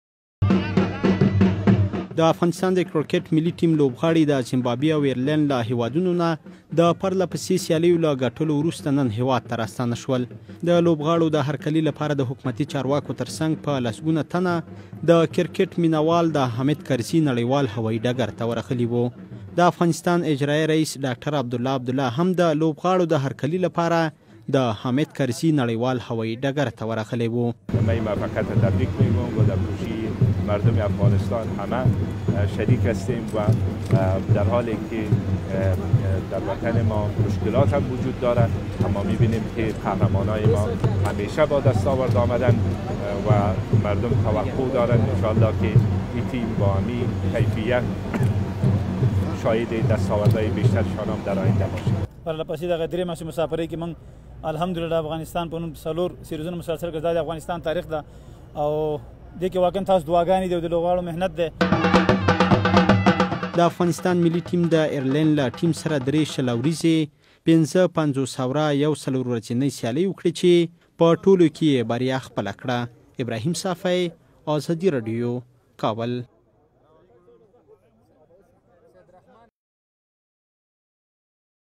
افغان کرکټ لوبډله هېواد ته را ستنه شوه
د افغانستان د کرکټ ملي لوبډله ایرلینډ ته په شل اوریزو، یو ورځنیو او څلور ورځنیو سیالیو کې له ماتې ورکولو ورسته نن کابل ته راستانه شوه. ویډیو او ایډیټ: تمیم اخګر